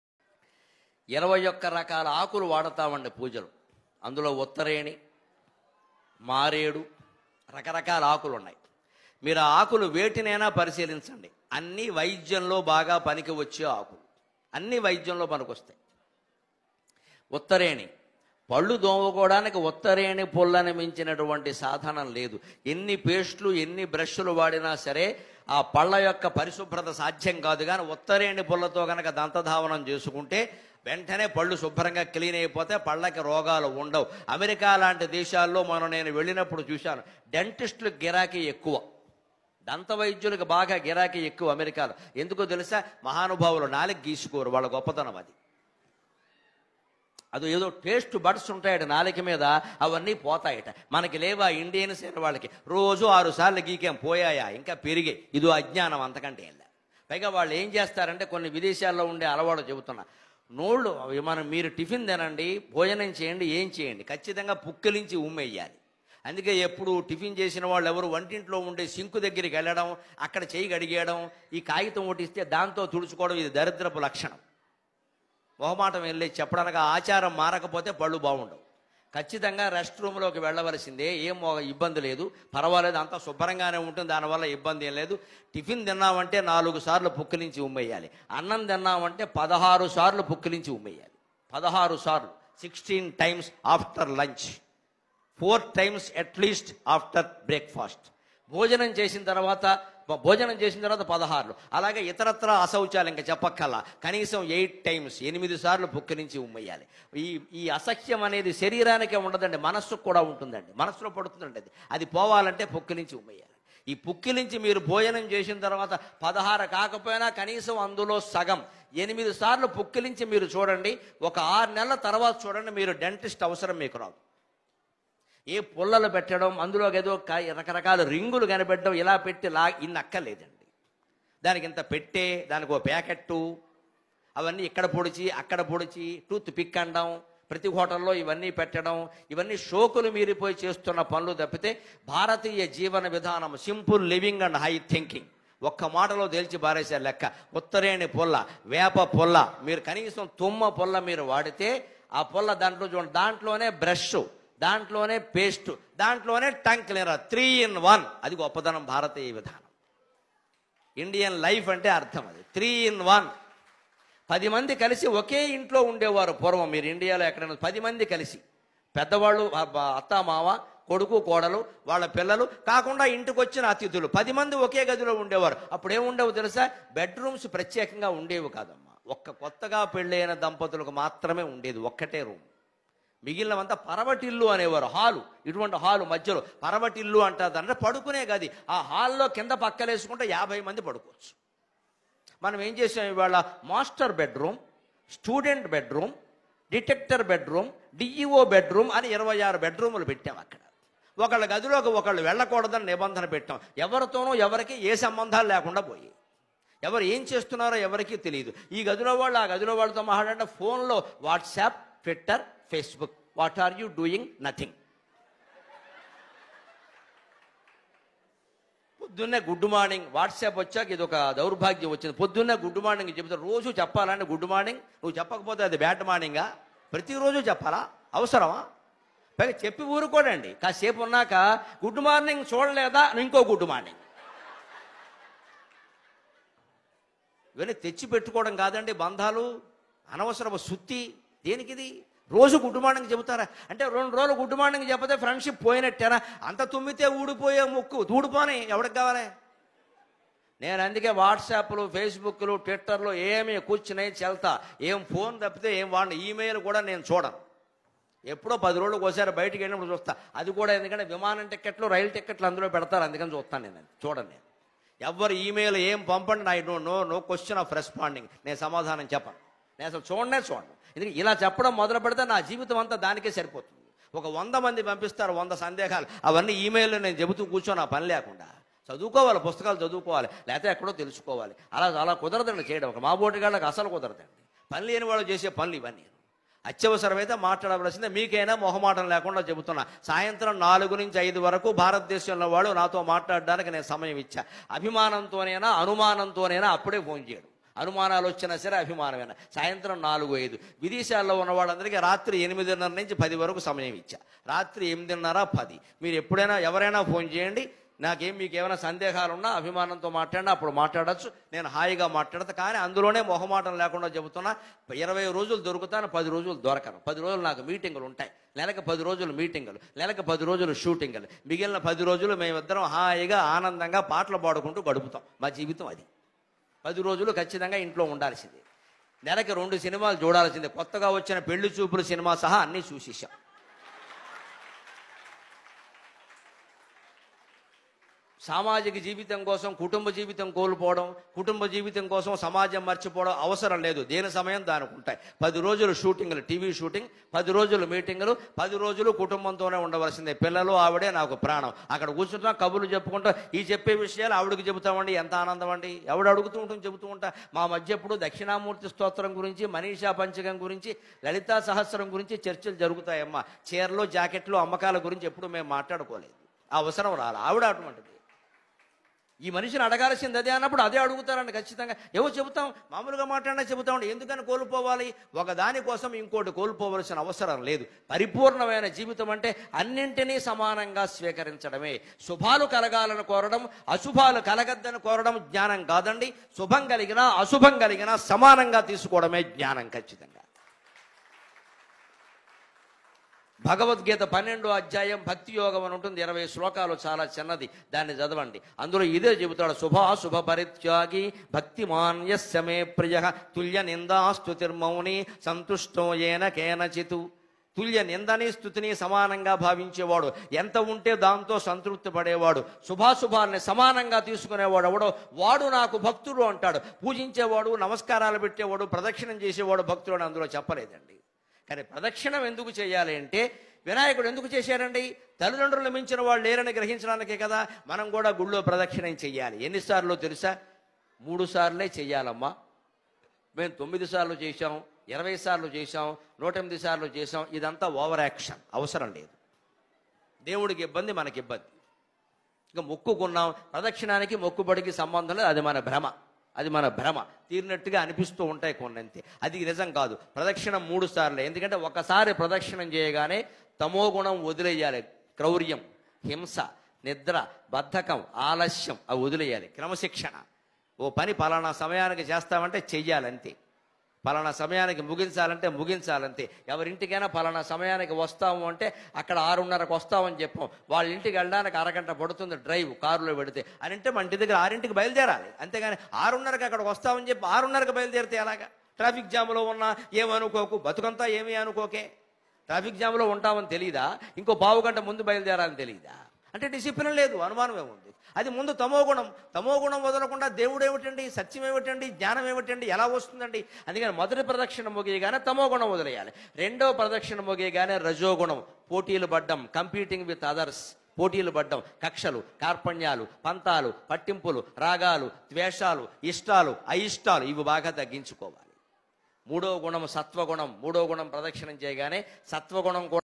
21 ఆకుల వలన కలిగే లాభాలు తెలుపుతున్న గరికపాటి spyder creations
21 ఆకుల వలన కలిగే లాభాలు తెలుపుతున్న గరికపాటి spyder creations